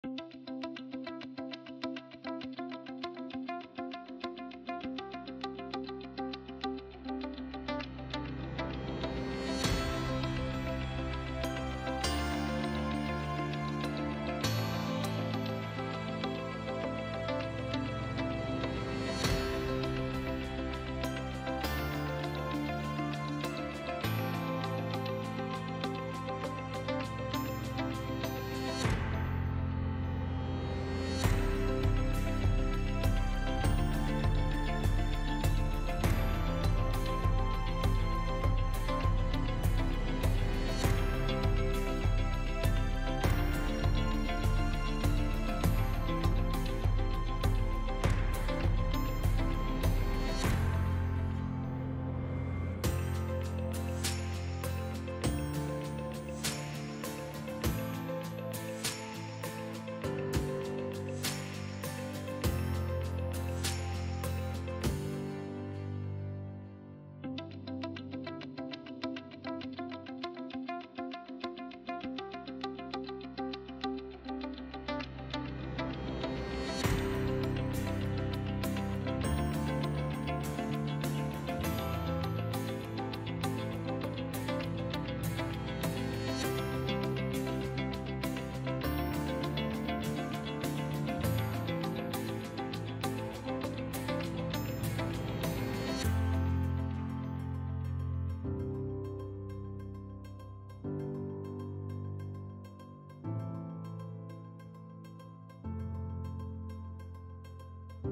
2018 Subaru Forester Myaree, Fremantle, Booragoon, Kardinya, Palmyra, WA 11014221
Silver Used 2018 Subaru Forester  available in Myaree, Western Australia. Servicing the Fremantle, Booragoon, Kardinya, Palmyra, WA area.

Melville Hyundai
170 Leach Hwy